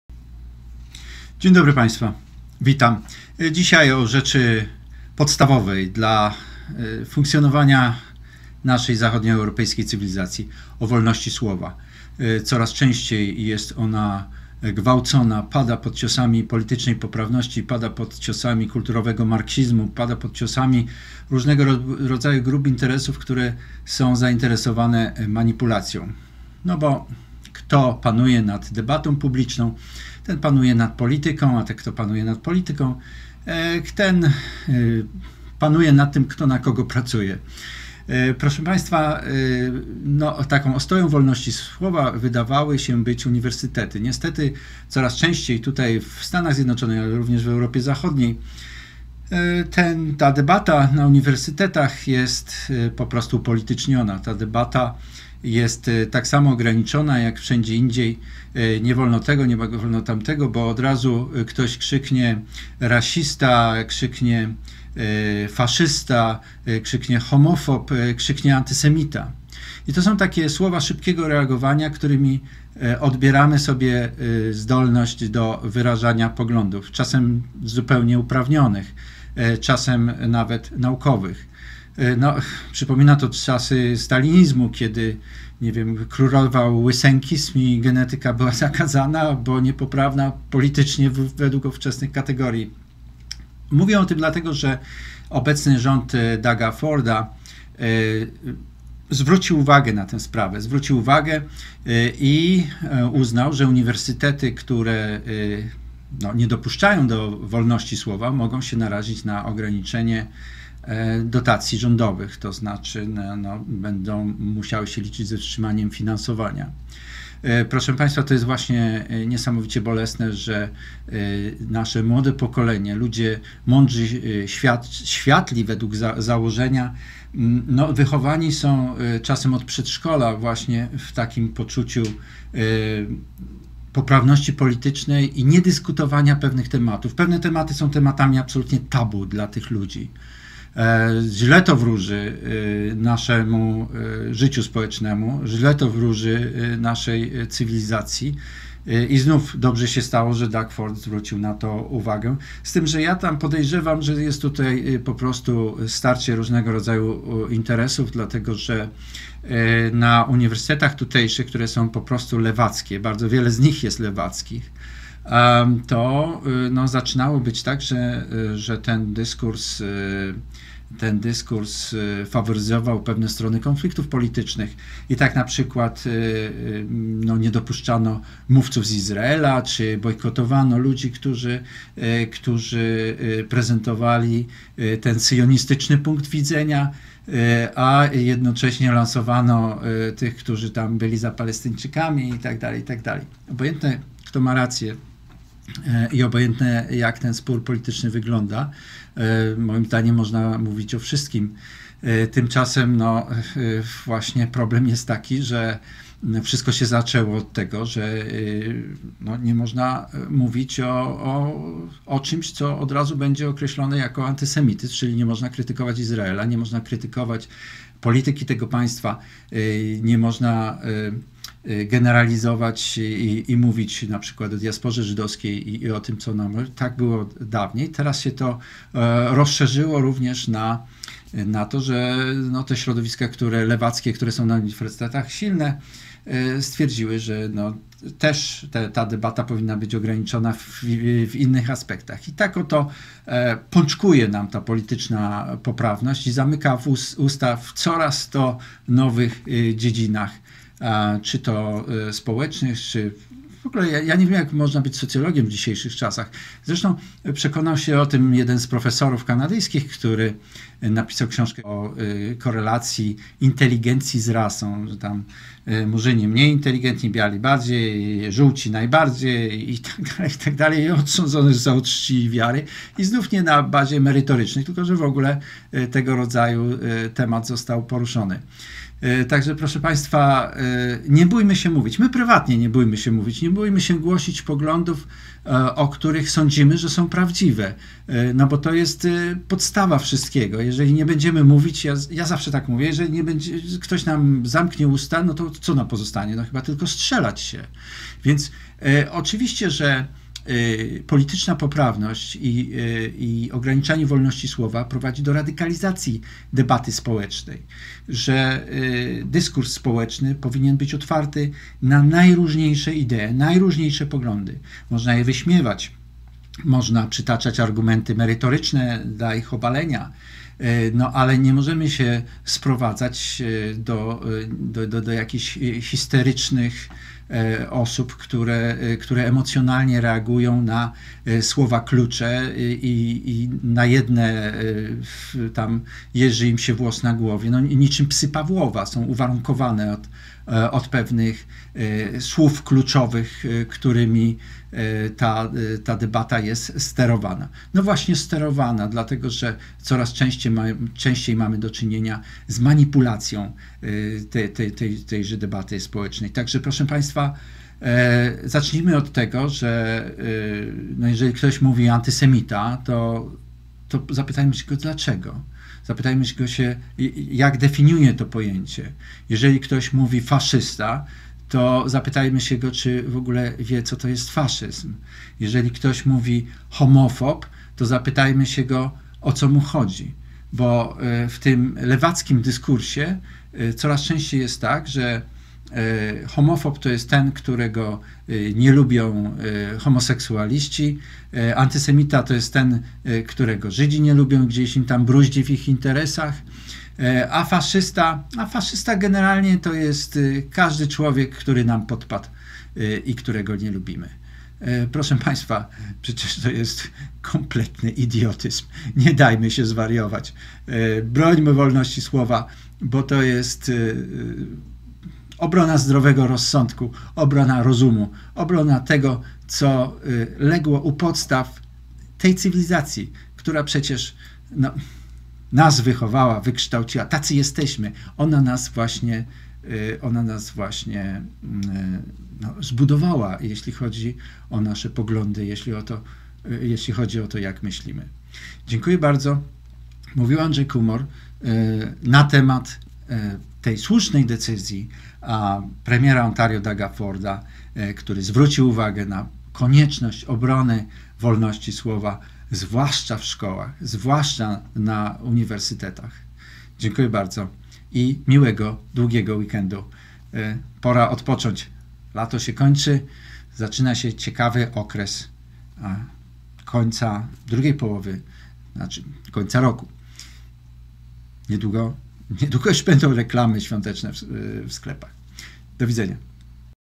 Premier Doug Ford broni wolnosci slowa
Premier Doug Ford zapowiedział, że szkoły wyższe, które nie będą szanować wolności debatowania mogą narazić się na utratę finansowania